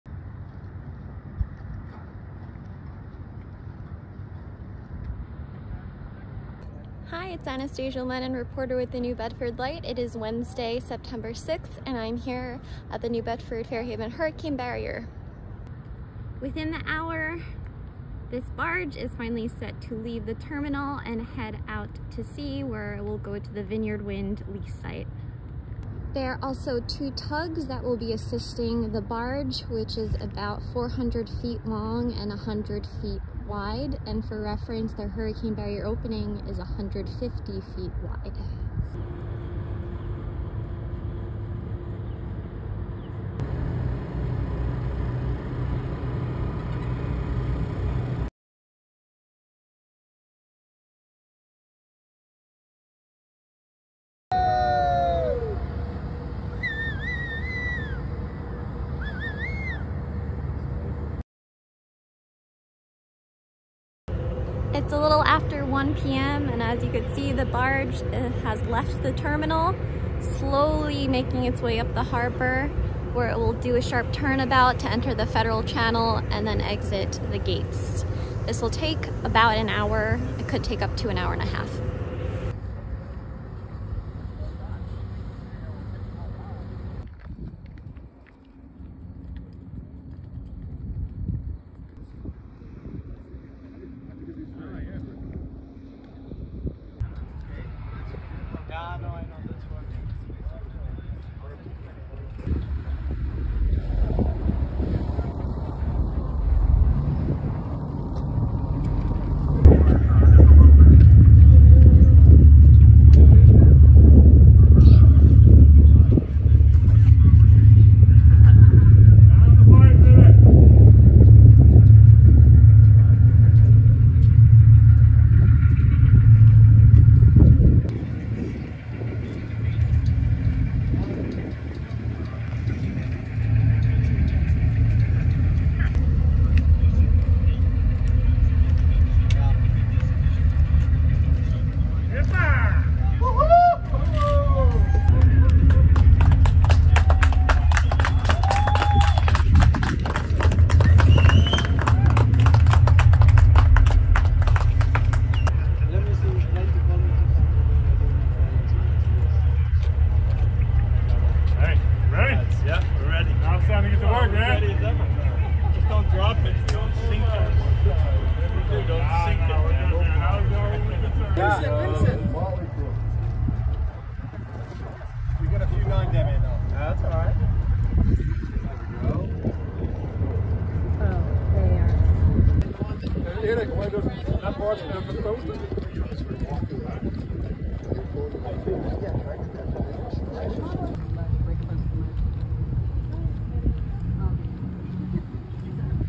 First assembled turbine parts leave New Bedford for Vineyard Wind site
Offshore wind reporter Anastasia E. Lennon was on site as a 100-foot-wide barge squeezed through the 150-foot-wide hurricane barrier opening on a hot and humid Wednesday, carrying three stacked blades, a nacelle, and two vertical towers — the parts for a single wind turbine. Full story